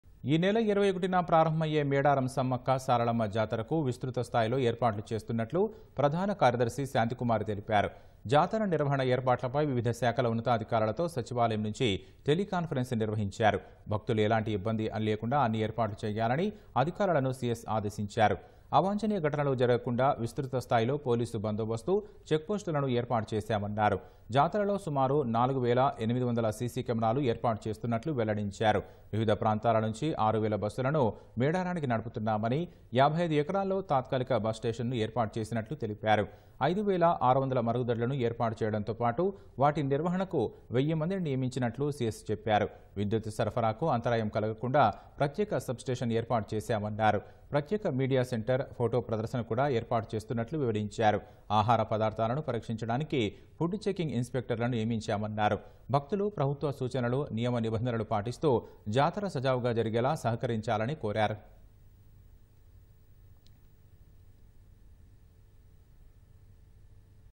4800 CCTV Camera Installed At Medaram Jatara | CS Shanthi Kumar | మేడారం జాతరకు పకడ్బందీ ఏర్పాట్లు
ఈనెల 21న ప్రారంభమయ్యే మేడారం సమ్మక్క, సారలమ్మ జాతరకు విస్తృత స్థాయిలో ఏర్పాట్లు చేస్తున్నట్లు ప్రధాన కార్యదర్శి శాంతికుమారి తెలిపారు. జాతర నిర్వహణ ఏర్పాట్లపై వివిధ శాఖల ఉన్నతాధికారులతో సచివాలయం నుంచి టెలీకాన్ఫరెన్స్ నిర్వహించారు. భక్తులు ఎలాంటి ఇబ్బంది అన్ని ఏర్పాట్లు చేయాలని అధికారులను సీఎస్ ఆదేశించారు. అవాంఛనీయ ఘటనలు జరగకుండా విస్తృత స్థాయిలో పోలీసు బందోబస్తు, చెక్ పోస్టులను ఏర్పాటు చేశామన్నారు. జాతరలో సుమారు 4 వేల 800 సీసీ కెమెరాలు  ఏర్పాటు చేస్తున్నట్లు వెల్లడించారు.

వివిధ ప్రాంతాల నుంచి ఆరు వేల బస్సులను మేడారానికి నడుపుతున్నామని..  55 ఎకరాల్లో తాత్కాలిక బస్ స్టేషన్ ను ఏర్పాటు చేసినట్లు తెలిపారు. 5 వేల 600 మరుగుదొడ్లను ఏర్పాటు చేయడంతో పాటు వాటి నిర్వహణకు వెయ్యి మందిని నియమించినట్లు సీఎస్ చెప్పారు. విద్యుత్ సరఫరాకు అంతరాయం కలుగకుండా ప్రత్యేక సబ్ స్టేషన్ ఏర్పాటు చేశామన్నారు. ప్రత్యేక మీడియా సెంటర్, ఫోటో ప్రదర్శన కూడా ఏర్పాటు చేస్తున్నట్టు వివరించారు. ఆహార పదార్థాలను పరీక్షించడానికి ఫుడ్ చెకింగ్ ఇన్ స్పెక్టర్లను నియమించామన్నారు. భక్తులు ప్రభుత్వ సూచనలు, నియమ, నిబంధనలు పాటిస్తూ జాతర సజావుగా జరిగేలా సహకరించాలని కోరారు.